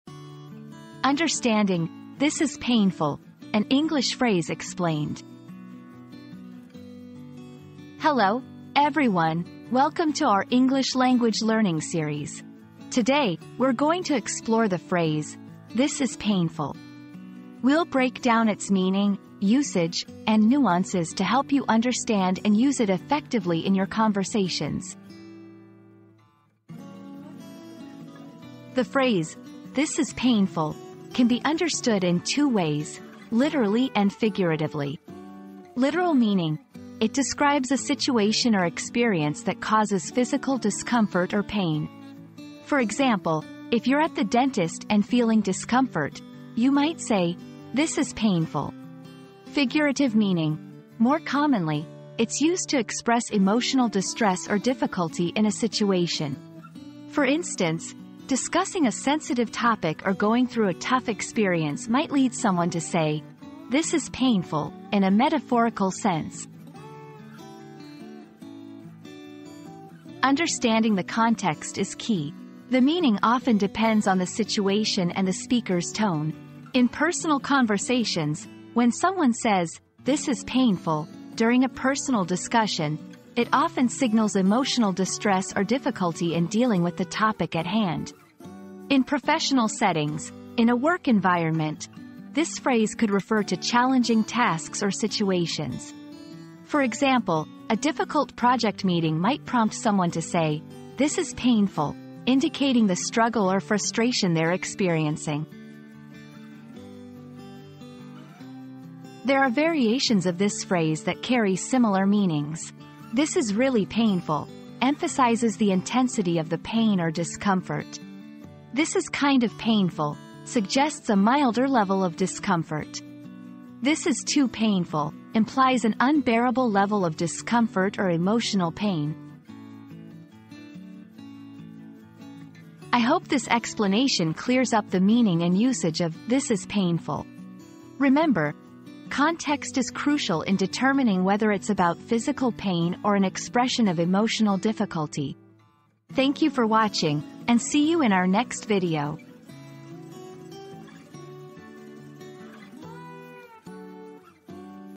Understanding "This is Painful": An English Phrase Explained
Decoding 'This is Painful': An Engaging Explanation of an English Phrase • Discover the meaning and usage of the popular English phrase 'This is Painful' in this captivating video. Gain insights into its cultural context and learn how to incorporate it effectively into your conversations.

00:00 • Introduction - Understanding "This is Painful": An English Phrase Explained
00:28 • Literal and Figurative Meanings
01:09 • Contextual Usage
01:48 • Variations of the Phrase